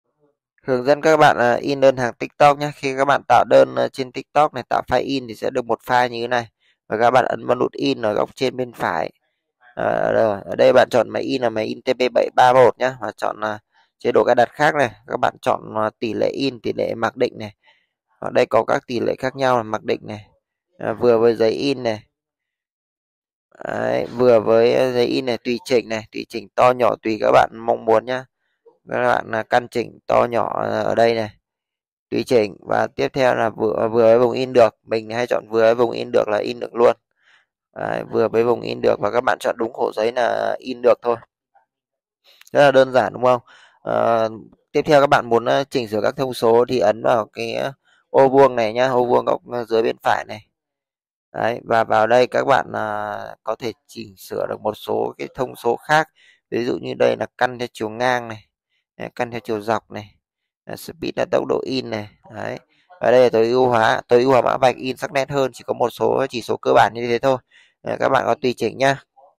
In Đơn hàng tiktok bằng Máy in nhiệt TP731
Máy in nhiệt TP-731 in được đơn tất cả các sản TMĐT, các bên vận chuyển, file pdf, excel, word…
Chiều rộng in từ 4cm-8cm
Máy in nhiệt TP-731 kết nối dây usb qua máy tính để in rất đơn giản
Không in được qua điện thoại di động
In nhanh sắc nét dễ dàng cài đặt và sử dụng